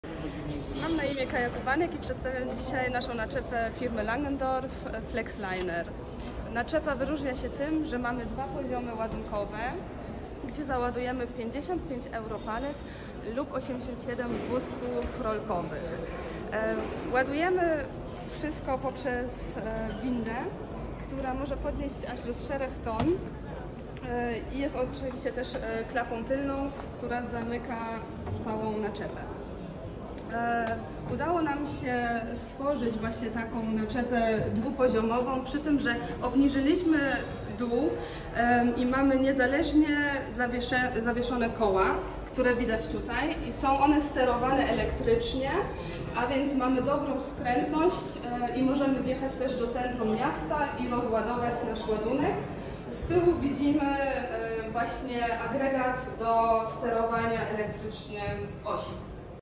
Kaja Kubanek o Langendorf Flatliner - nagrodzony w konkursie Trailers Innovation 2023 🏆
Na targach Translogistica Poland mieliśmy przyjemność spotkać Kaję Kubanek z Langendorf, która opowiedziała nam o innowacyjnym modelu Langedorf Flatliner. 🚛

🏆 Dwuosiowa konstrukcja i technologia osi  kierowanych w naczepie zostały nagrodzone w tym roku na targach IAA Transportation w konkursie Trailer Innovation 2023!

SPECYFIKACJA:

◻️ Objętość: 98 m3
◻️ Szerokość wewnętrzna: 2460 mm / 1620 mm pomiędzy kołami
◻️ Wysokość wewnętrzna: 2 x 1820 mm
◻️ Długość wewnętrzna: 13610 mm góra / 9750 mm dół
◻️ Podłoga: Profile aluminiowe 45 mm
◻️ Ściany boczne: Panel 25 mm, aluminium
◻️ Masa własna: 10 500 kg
◻️ Rama: Stalowa
◻️ Felgi: Stalowe 11,75 x 22,5 ET 45
◻️ Oświetlenie: LED
◻️ Osie: BPW Eco Plus/niezależne zawieszenie kół firmy Langendorf
◻️ Elektroniczne sterowanie osi: VSE
◻️ Układ hamulcowy: WABCO EBS
◻️ Opony: rozmiar 385/55 R22,5
◻️ Winda: MBB 4000V/siła podnoszenia 4 000 kg, dł. 3 600mm/szer. 2 082 mm